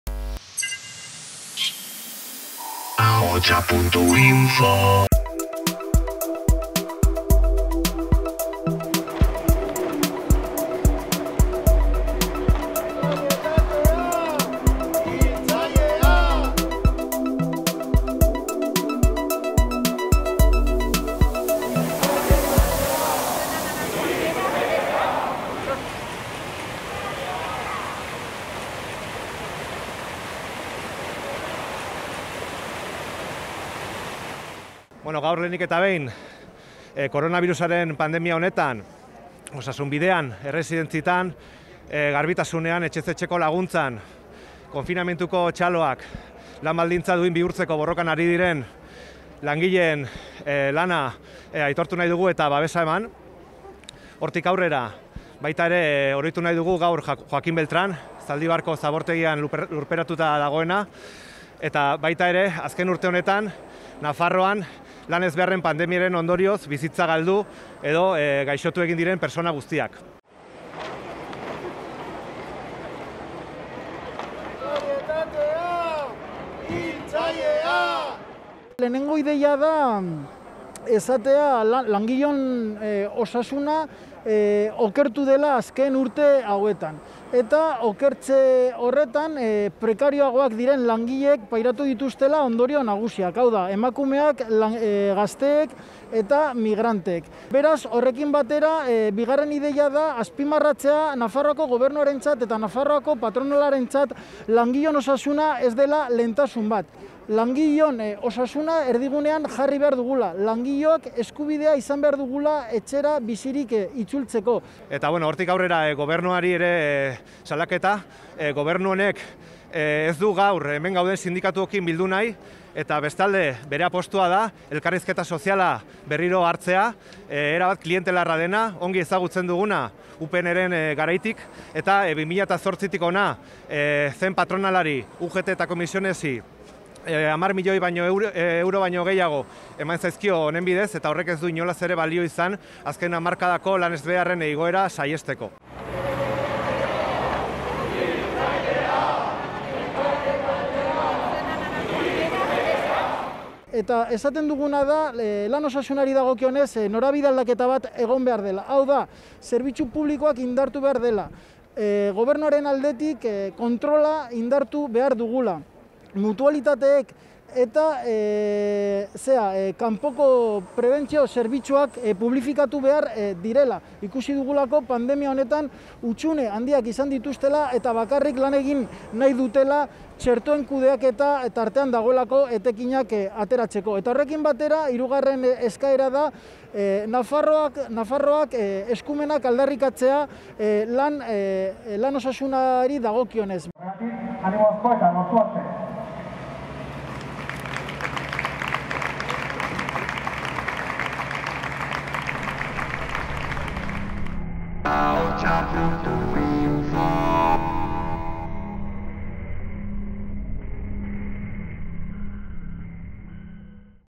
Lan osasunean prebentzio eta kontrol neurriak exijitu dizkiote sindikatuek patronalari eta Gobernuar
Gaur Laneko Segurtasun eta Osasunaren Nazioarteko Eguna da, eta Nafarroako sindikatuek aprobetxatu dute istripuak eta lanaren ondorioz langileek pairatzen dituzten gaixotasunak salatzeko Iruñean egindako manifestazio batekin. Egoera jasangaitza dela azpimarratu dute, eta prebentzio neurriak eta kontrol publiko geihago eskatu dute.